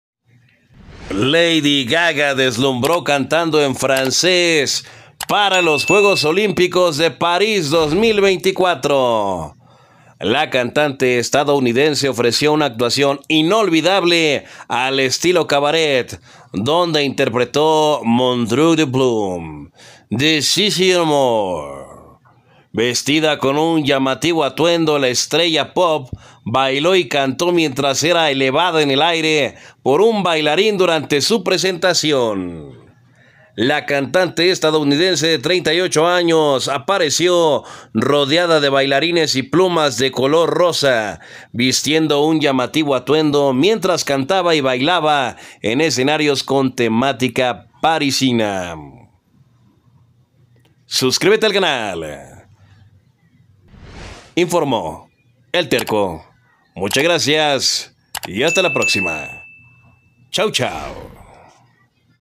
LADY GAGA OFRECIO UNA ACTUACIÓN INOLVIDABLE EN LA INAUGURACIÓN DE LOS JUEGOS OLIMPICOS DE PARIS 2024
terco informa acerca de las tendencias más importantes del mundo.

regalamos dinero entra aquí
Hola el terco te ha enviado MX$30.00 de descuento para tu primer pedido.